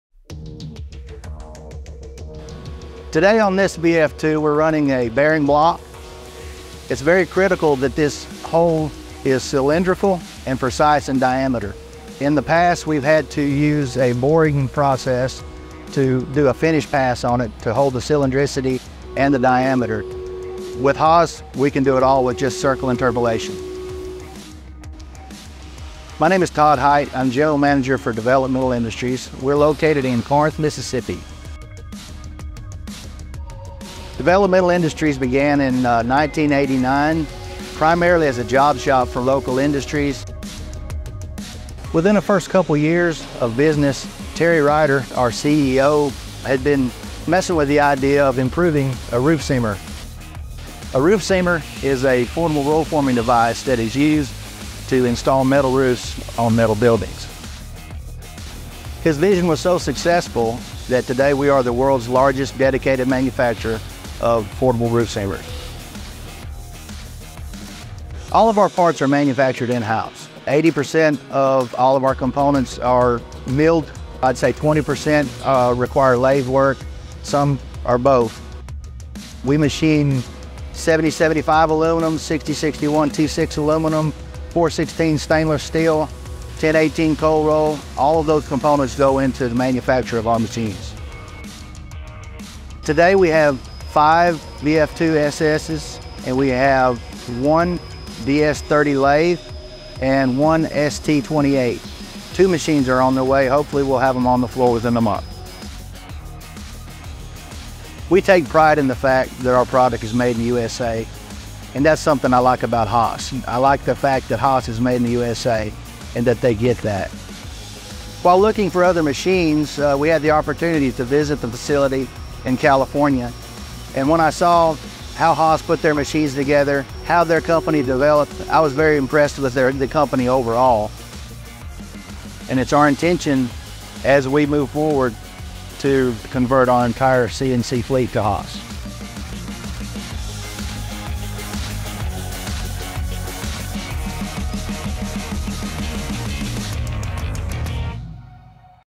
Machining Critical Parts for Roof Seamers in Haas CNC's in Mississippi
Developmental Industries began as a general-purpose job shop and today is the world’s largest dedicated manufacturer of Roof Seamers. Their popular roof seamer is manufactured in their Mississippi plant, where they have 5 Haas CMC mills and 2 CNC lathes machining all critical components.

In this Haas INSIDER video, Todd Hight, General Manager at Developmental Industries shares details about a bearing block they machine on their Haas VF-2SSYT, that previously required a boring operation. Hight also shares how a visit to the Haas factory (in 2019) led to their first Haas purchase.